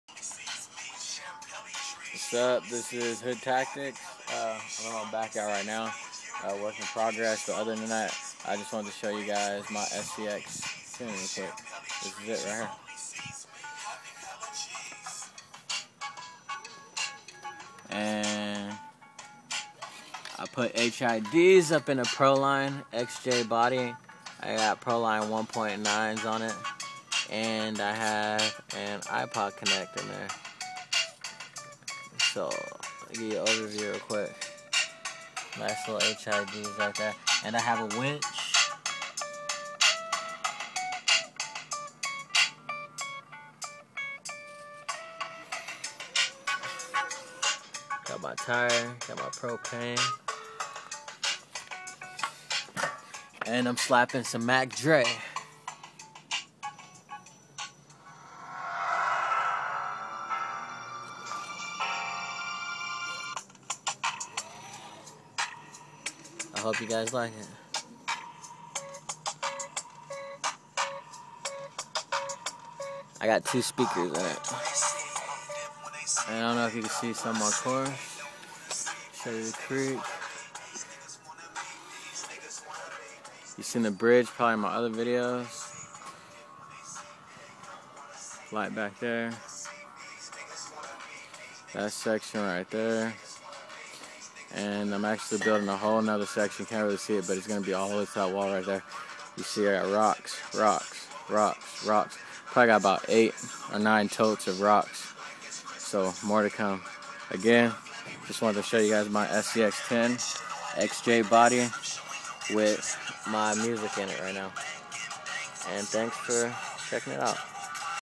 SCX10 xj with iPOD connect
Quick look at my axial scx10 with iPod hook up and two speakers, hid, winch.